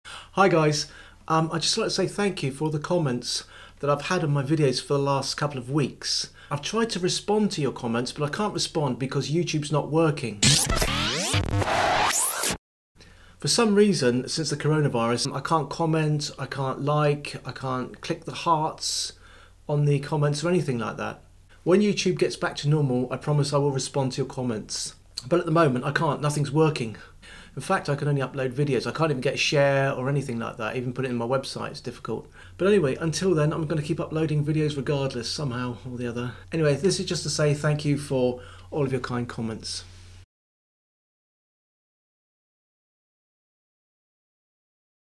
Why I haven't replied to any comments etc on Youtube?
I can't reply to your comments because youtube isn't working properly for my channel at the moment, I would imagine that this is due to the Coronavirus. I will reply to all of your comments when youtube gets back to normal.